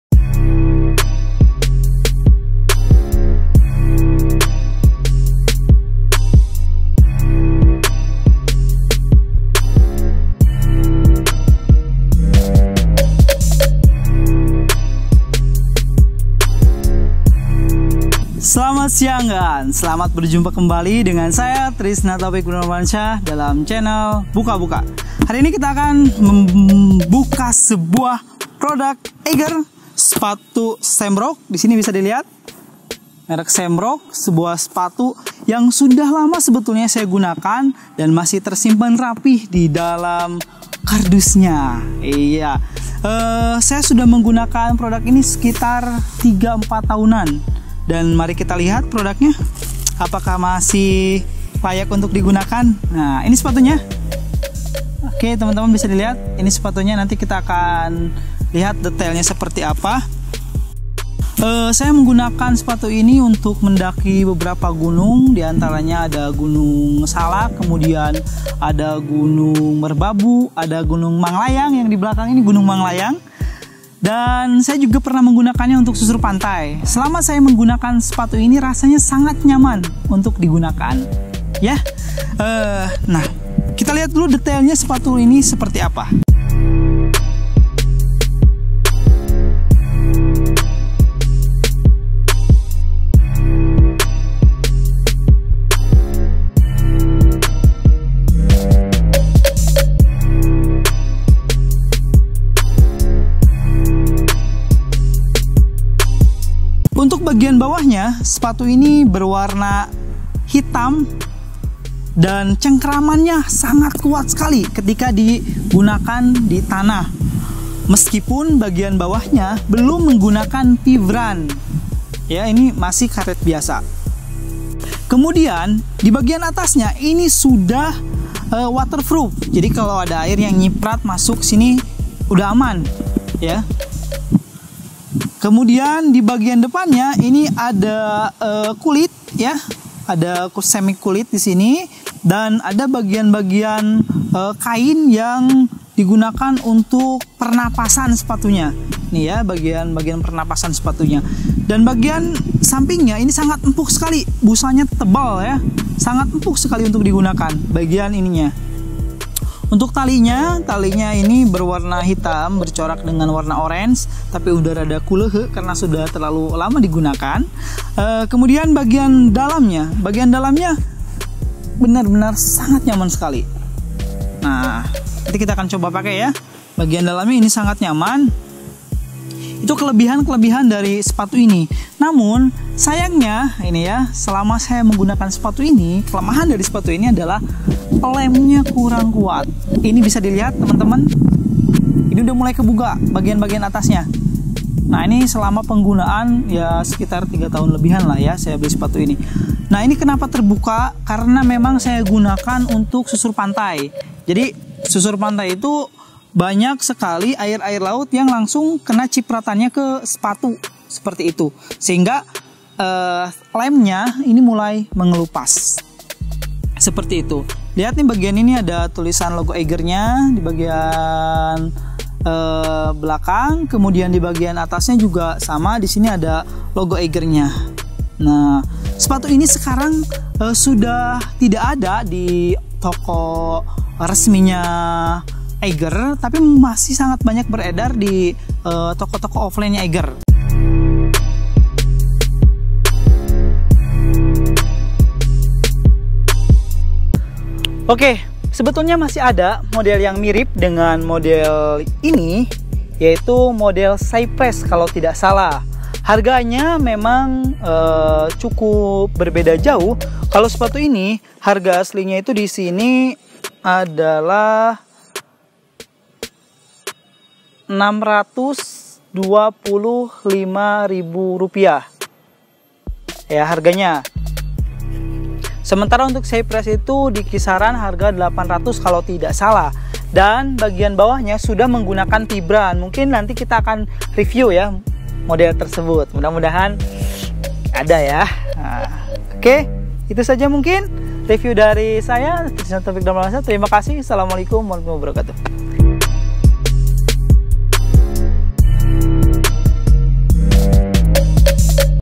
Buka Buka #47 Eiger Mid Boot Shamrock versi lama dari EIGER CYPRESS TX SHOES
Salah satu sepatu dengan style mountaineering dikembangkan EIGER, untuk memenuhi kebutuhan para petualang yang ingin tampil stylish untuk pemakaian daily, meski dirancang seperti sepatu technical boots, namun sepatu ini tidak direkomendasikan untuk penjelajahan medan ekstrim, namun masih bisa digunkan untuk melintasi medan tanah,lumpur, bebatuan,kerikil,rerumputan dengan jarak dan waktu tempuh yang tidak terlalu sulit (hiking ringan). Didukung dengan material upper Suede leather, dikombinasikan dengan polyester fabric untuk karakter kuat ditambah Booties construction sytem dipastikan Waterproof. Sepatu ini juga dilengkapi dengan Plate shank dibagian midsole agar bagian outsole lebih kaku dan kokoh untuk menunjang kaki agar tidak cepat lelah.

Shot on :Eos M
Videographer : Dzaka